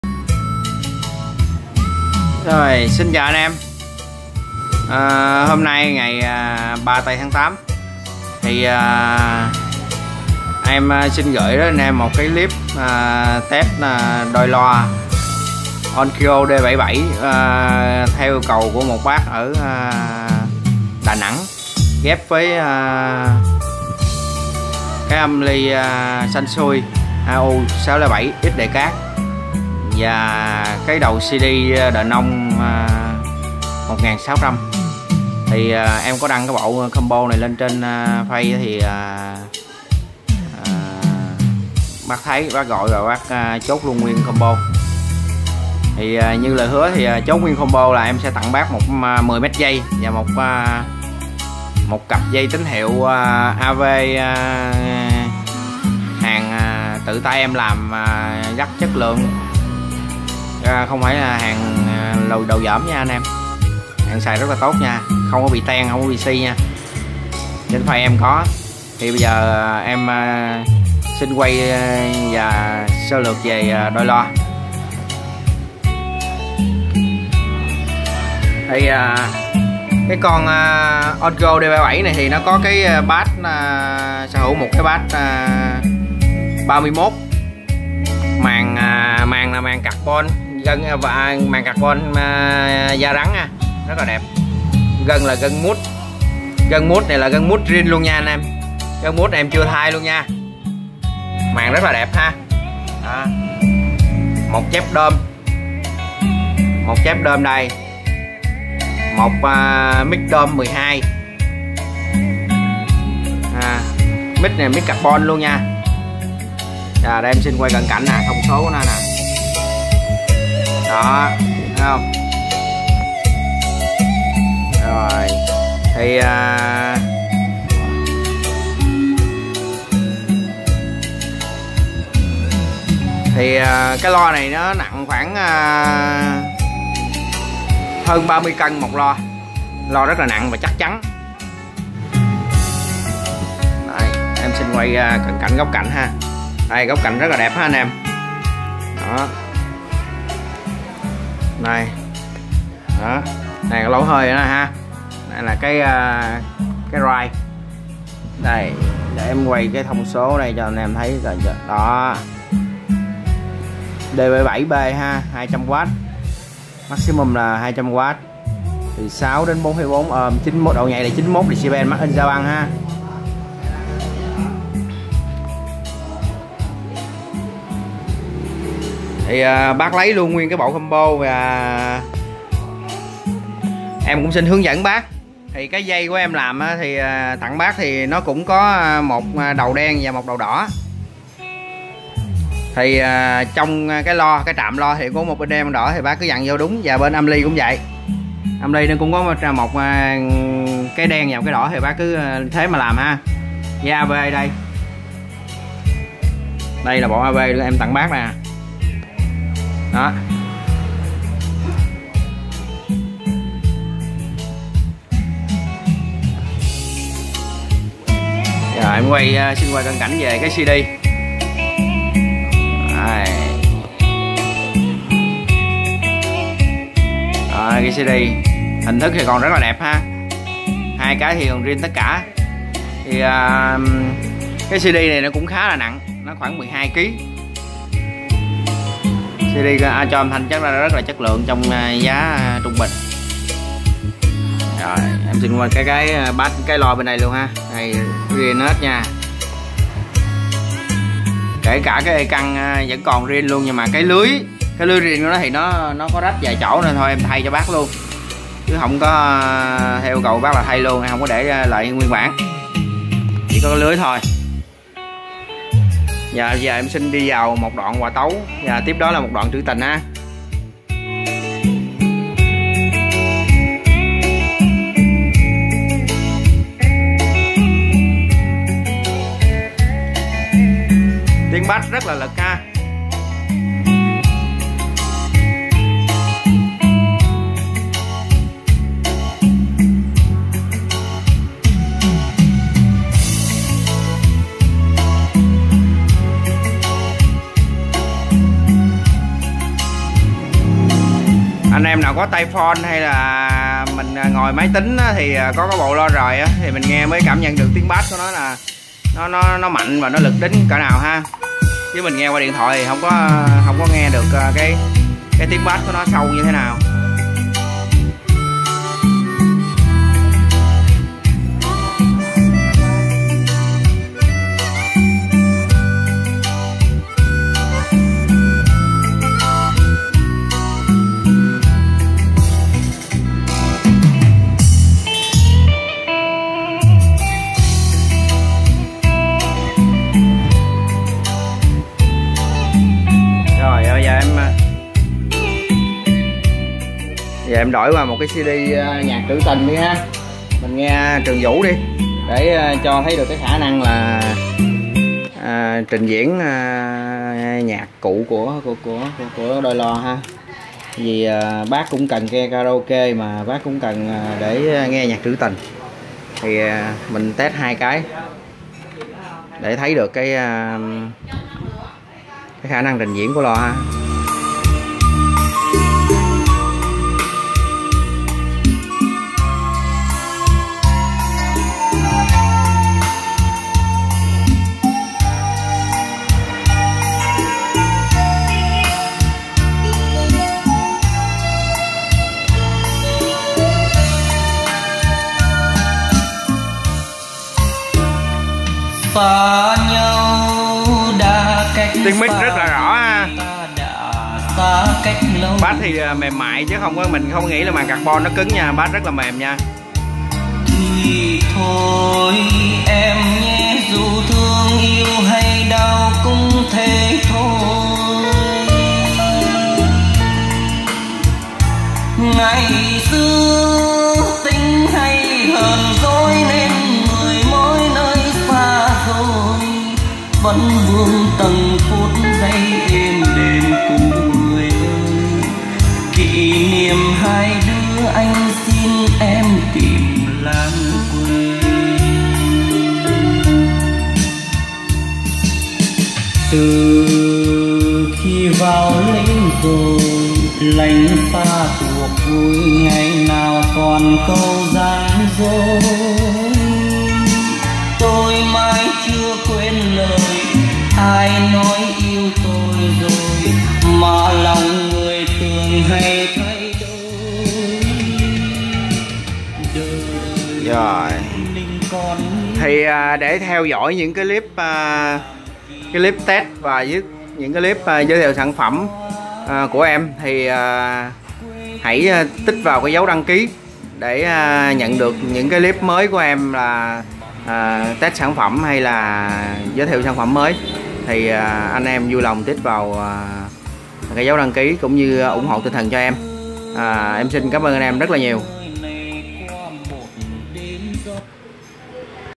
03/08 Test ONKYO D77 + SANSUI AU-D670XDECADE + DCD DENON 1600 gửi bác Minh Đà Nẵng. LH 016436.12345
Combo 20t500 ngàn tặng 10m dây loa và dây AV DIY xin